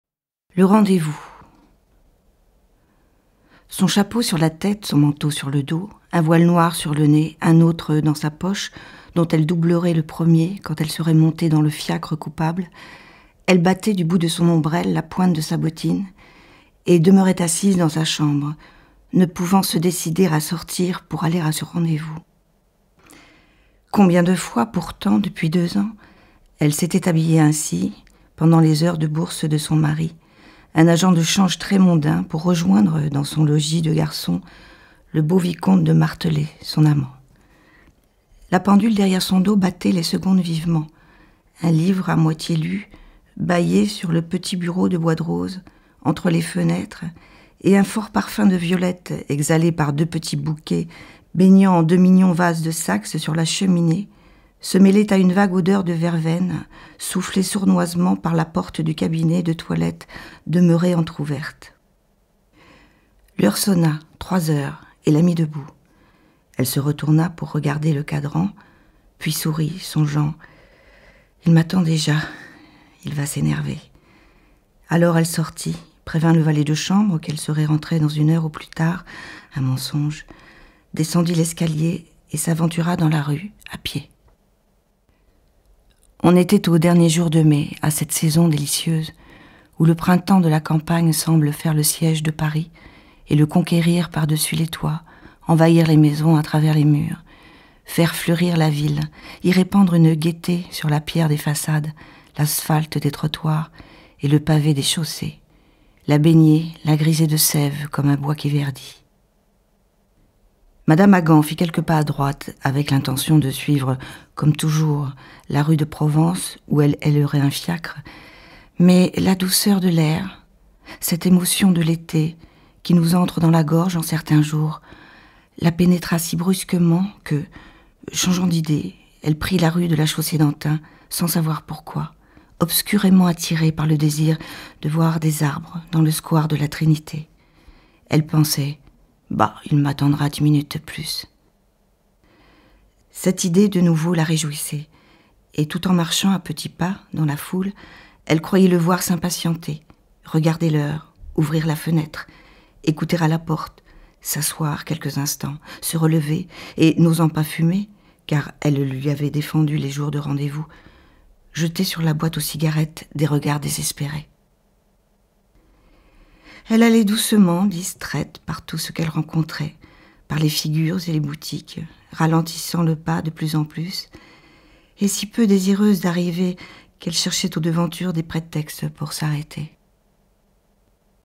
Le Rendez-vous: Début
Le Rendez-vous: Début · Miou Miou

Maupassant et les femmes : Nouvelles

Narrator: Miou Miou
Composer: Guy de Maupassant
Author: Guy de Maupassant